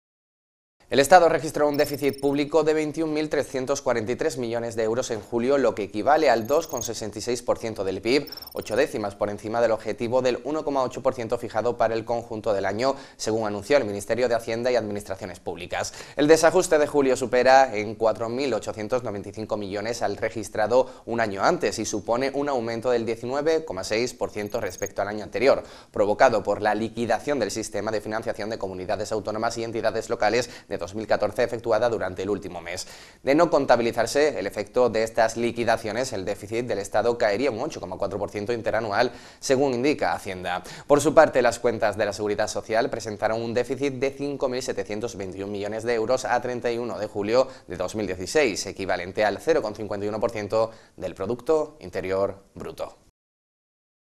Málaga 24h TV - El Estado eleva su déficit al 2,6% del PIB en julio
El Estado registró un déficit público de 21.343 millones de euros en julio, lo que equivale al 2,66% del PIB, ocho décimas por encima del objetivo del 1,8% fijado para el conjunto del año, según anunció el Ministerio de Hacienda y Administraciones Públicas.

El desajuste de julio supera en 4.895 millones al registrado un año antes y supone un aumento del 19,6% respecto al año anterior, provocado por la liquidación del sistema de financiación de comunidades autónomas y entidades locales de 2014 efectuada durante el último mes.

De no contabilizarse el efecto de estas liquidaciones, el déficit del Estado caería un 8,4% interanual, según indica Hacienda.

Por su parte, las cuentas de la Seguridad Social presentaron un déficit de 5.721 millones de euros a 31 de julio de 2016, equivalente al 0,51% del Producto Interior Bruto.

English: The State recorded a deficit of 21.343 million euros in July, equivalent to 2.66% of GDP, eight tenths above the target of 1.8% set for the whole year, announced the Ministry of Finance and Public administrations.

July mismatch exceeds 4,895,000 to a year ago and an increase of 19.6% over the previous year, caused by the liquidation of the system of financing of autonomous communities and local entities 2014 made during the last month.

Not accounted for the effect of these settlements, the government deficit would fall by 8.4% year on year, according to Finance.

Meanwhile, the Social Security accounts showed a deficit of 5.721 million euros to 31 July 2016, equivalent to 0.51% of gross domestic product.